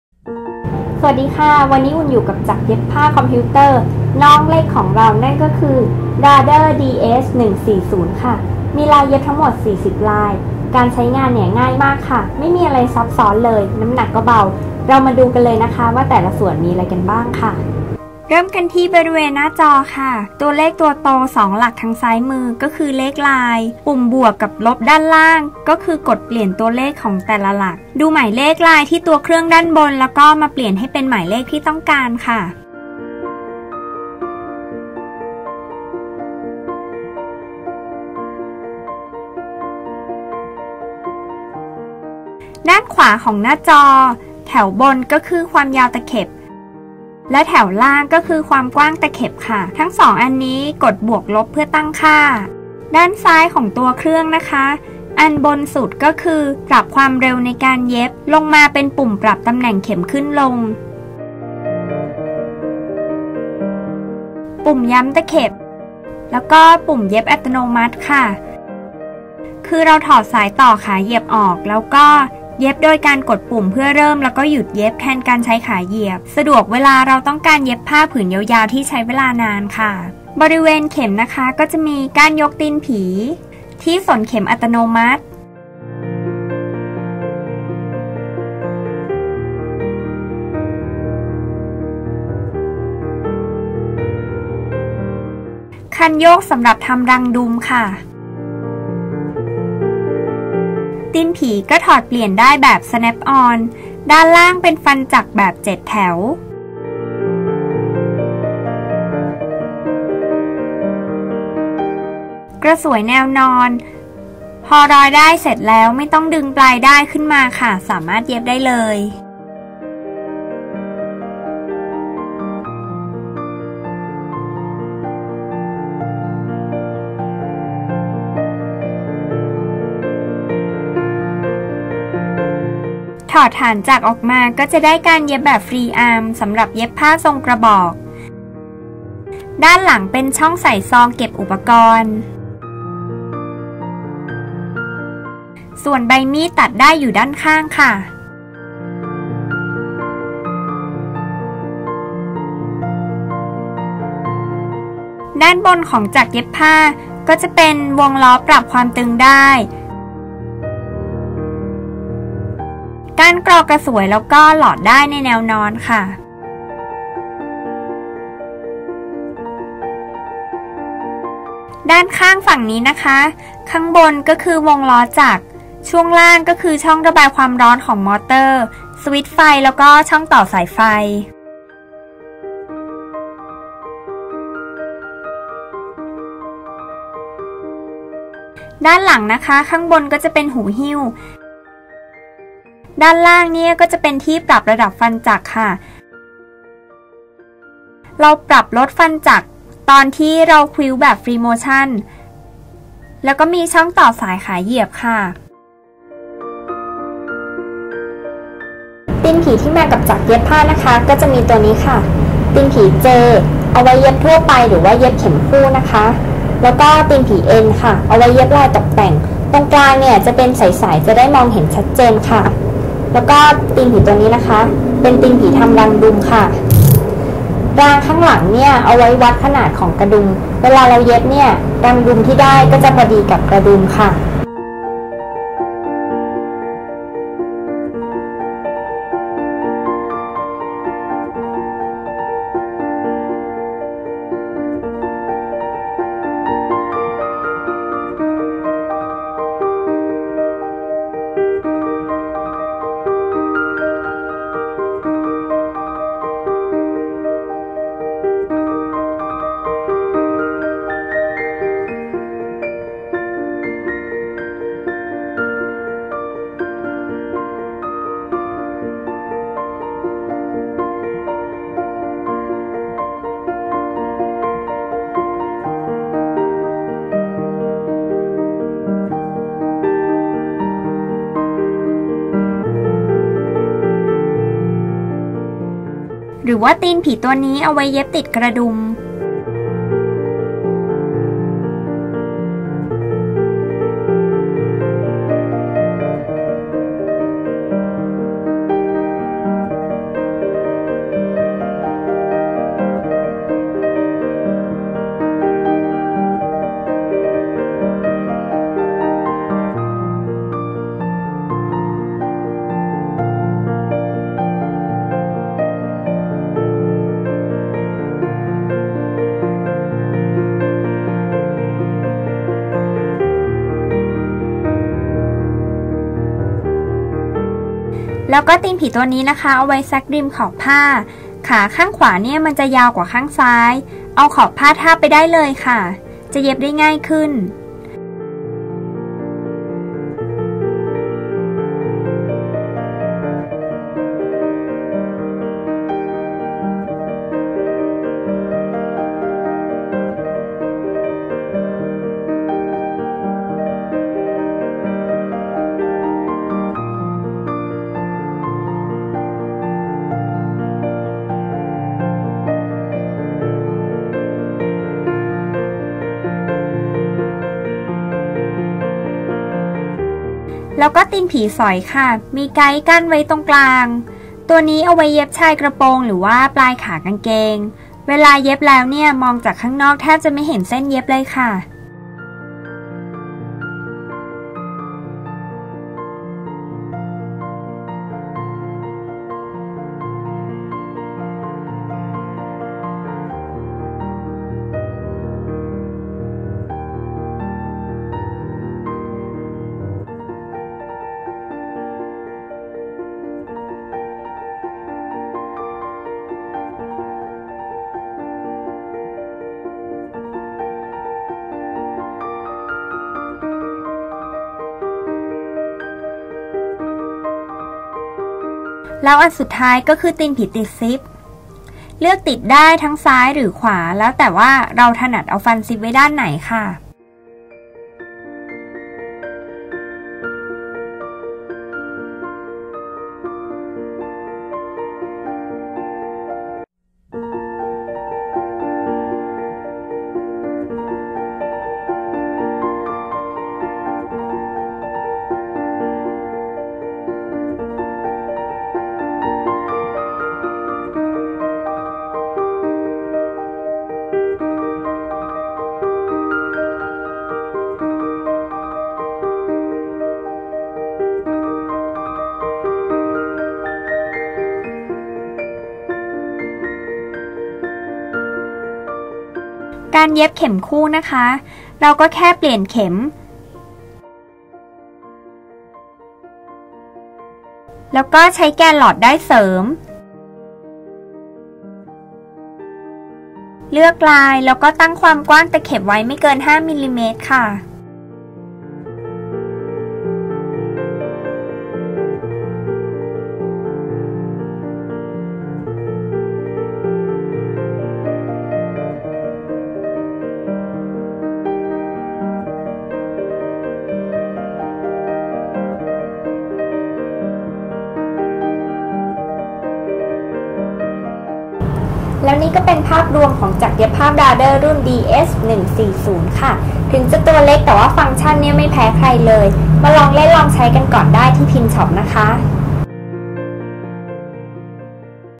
Review brother DS 140 by PINN SHOP
คลิปวีดีโอรีวิวจักรคุณภาพ brother DS-140 by PINN SHOP
สนใจสอบถามรายละเอียดเพิ่มเติมได้ที่ร้าน PINNSHOP โทร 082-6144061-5